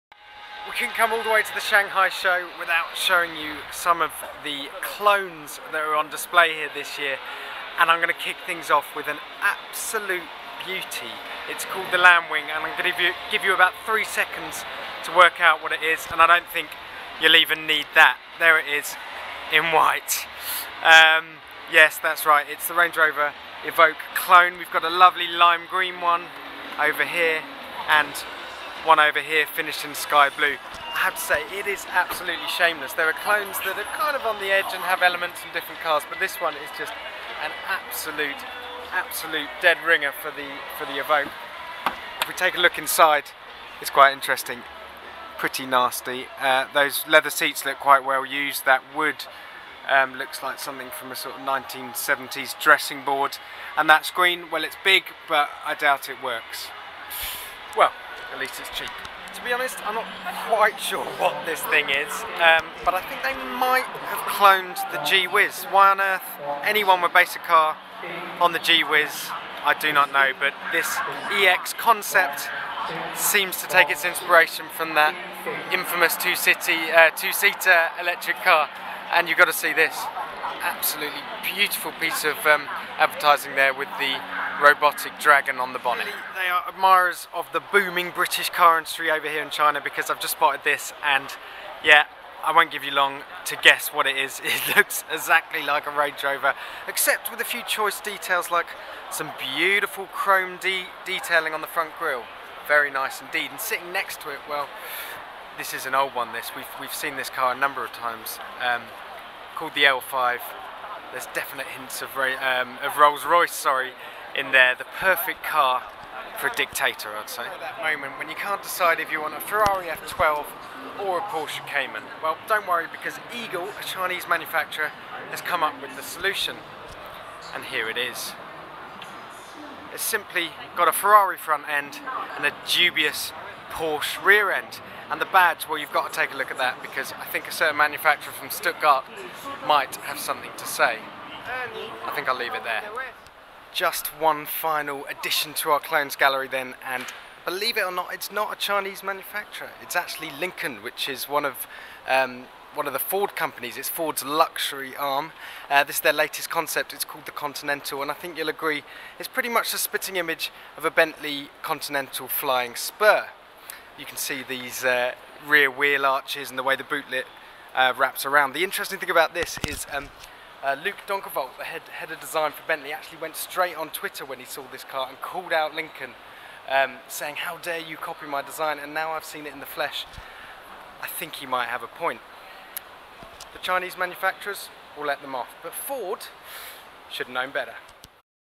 Attack of the clones: funny copycat cars from Shanghai
As China’s car market booms, we look at the shameless lookalikes Chinese car makers are showing off at the Shanghai Motor Show.